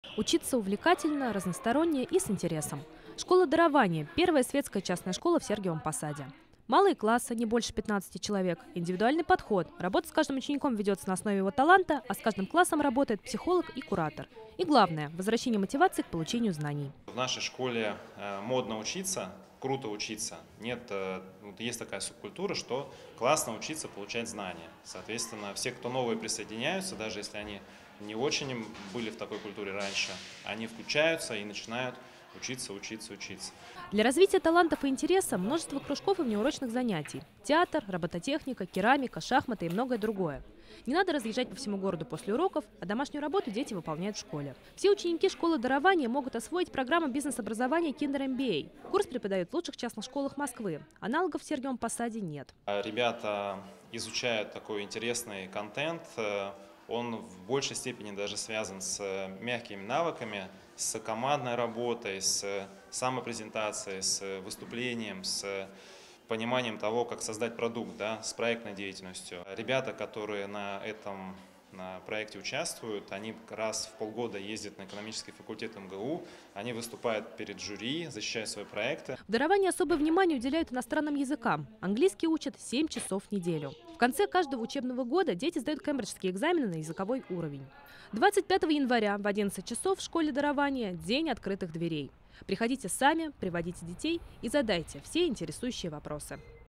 Школа «Дарование» приглашает на День открытых дверей 25 января | Новости | ТВР24 | Сергиев Посад
Учиться с увлечением, разносторонне, интересно. Школа «Дарование» – первая светская частная школа в Сергиевом Посаде. Здесь малые классы – не больше 15 человек, индивидуальный подход – занятия с учеником ведутся на основе его таланта, а с каждым классом работают психолог и куратор. И главное – здесь возвращают мотивацию к получению знаний.